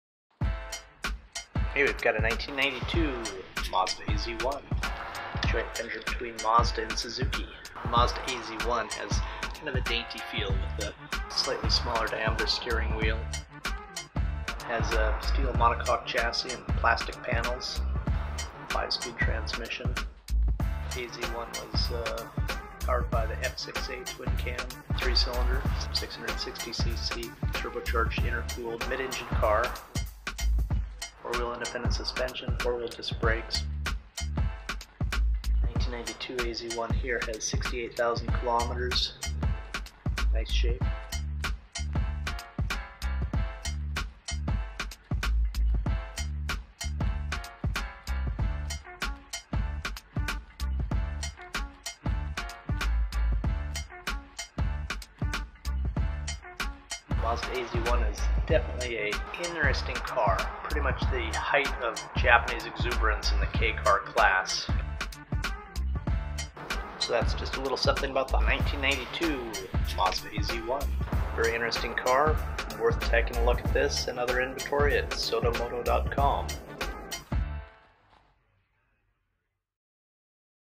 SODO Moto | Mazda AZ-1
SODO Moto Seattle | Mazda AZ-1 PG6SA-101962 for sale Autumn, 2018 in Seattle WA.